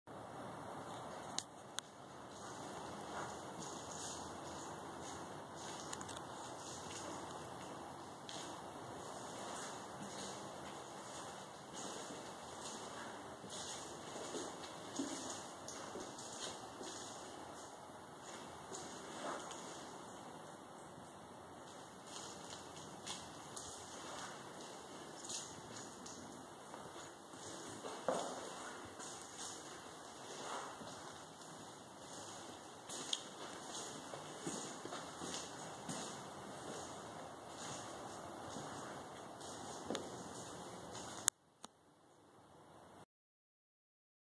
Richmond Home for Sale - 327 S 35th St, Richmond: Garage
327 S 35th St, Richmond: One car attached garage with new garage door and more space enough as a bedroom. For more information or other properties, please